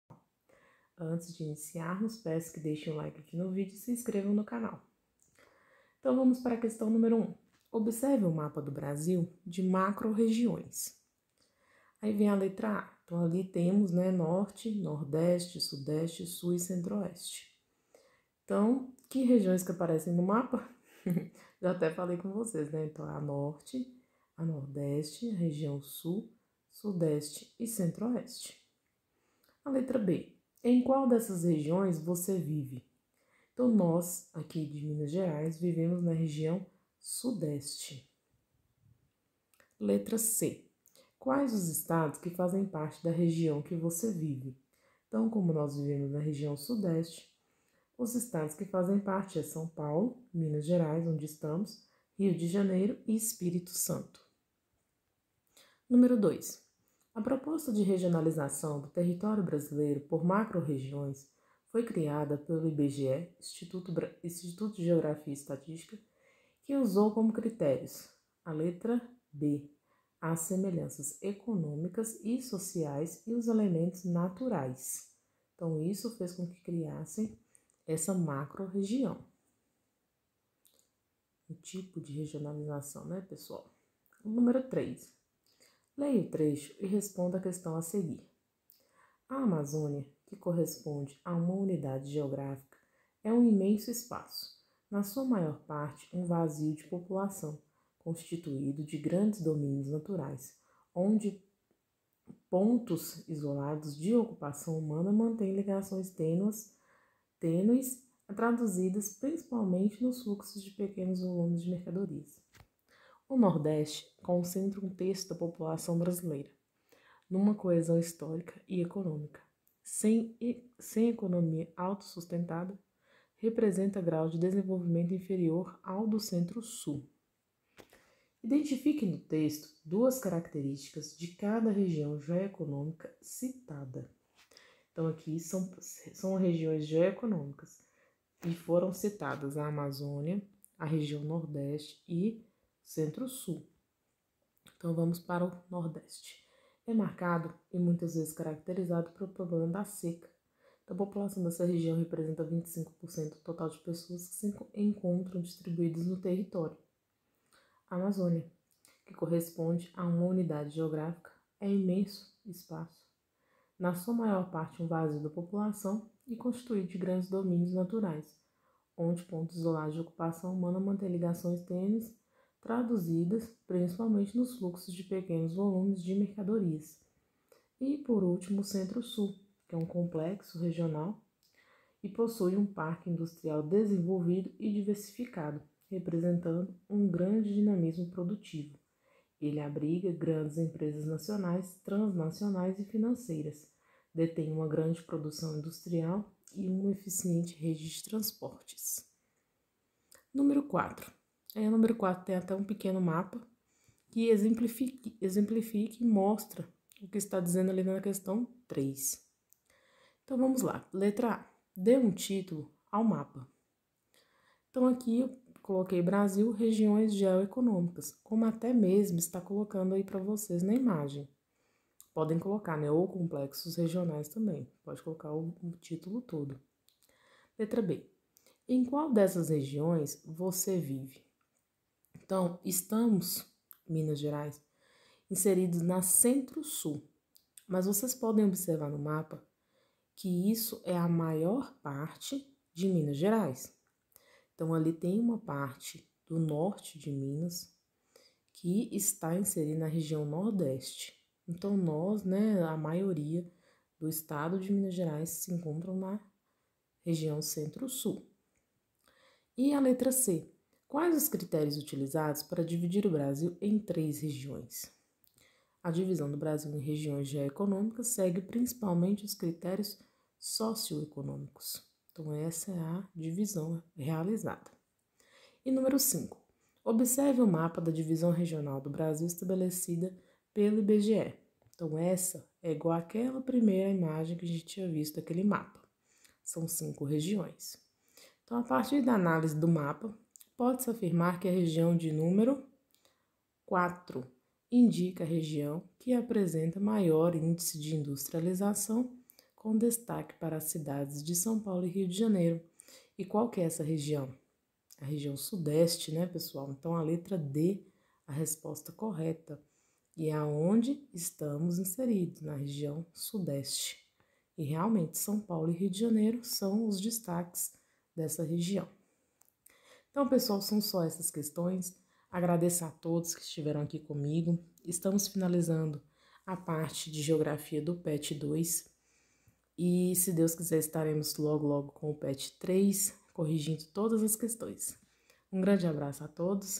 Correção - PET 2/2021 - Geografia 7° ano - Semana 6 [GABARITO]
Correção das questões do PET – Volume 2 - 2021, da Semana 6, do 7º Ano – Ensino Fundamental – Geografia (Minas Gerais)[GABARITO]

Faça parte da nossa família! Assim podemos levar nosso conteúdo e ajudar muitas outras pessoas!